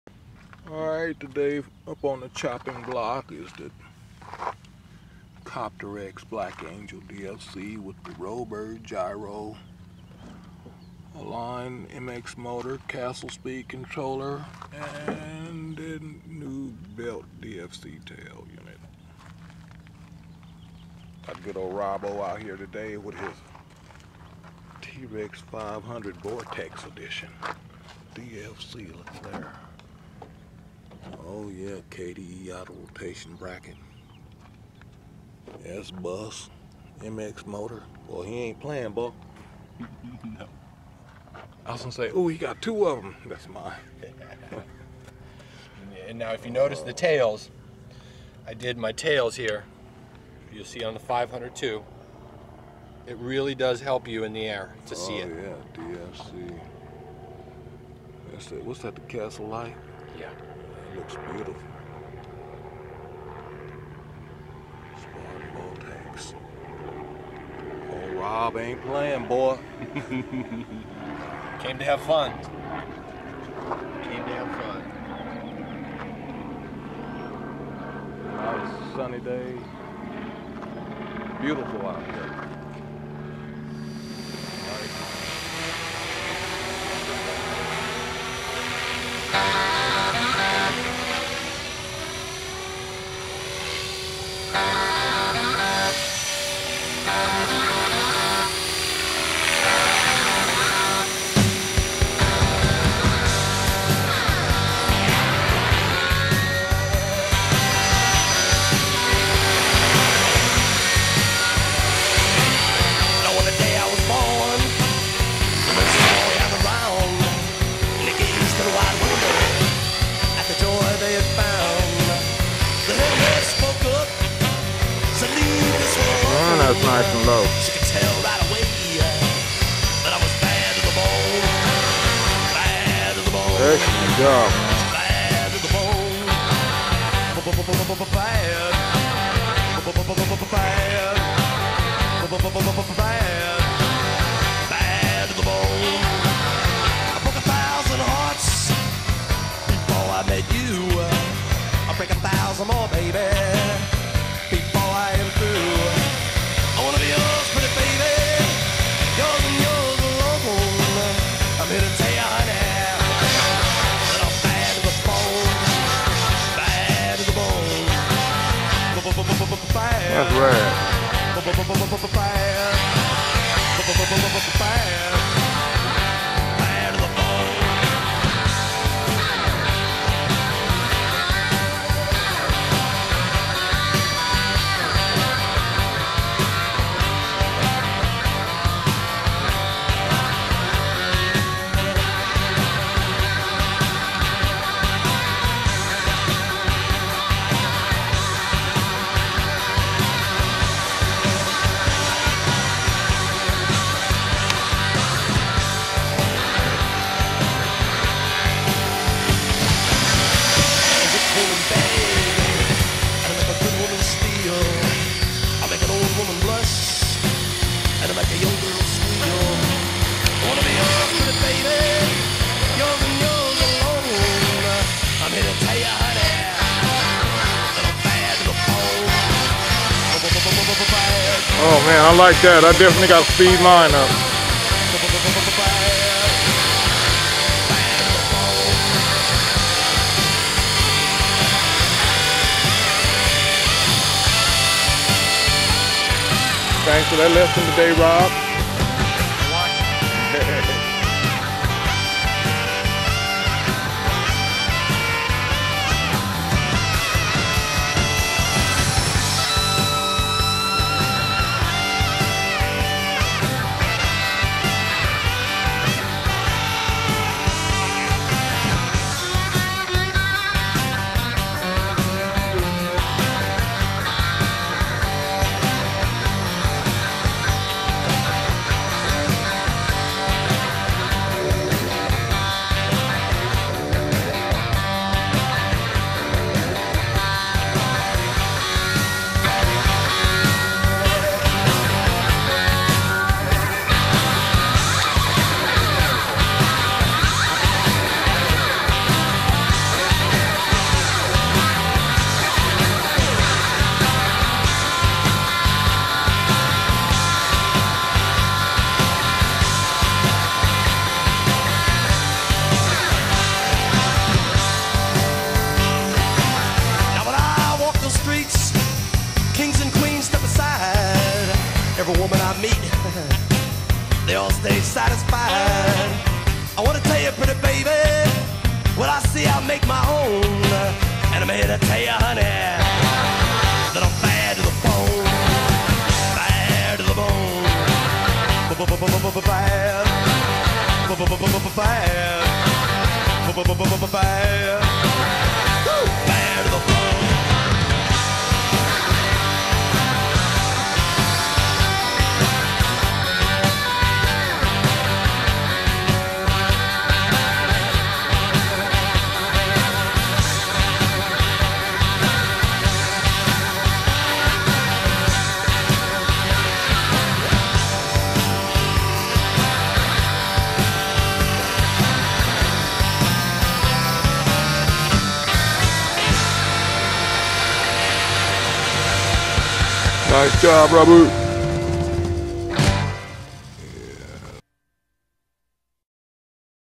Big Rob is bad to the bone doing 3d trex 450 and 500
i was Roberts first mentor now he is my mentor lol
i mentor them then they come back and mentor me guess im getting old but it feels good to share and be loved by all of you thanks for watching love you all .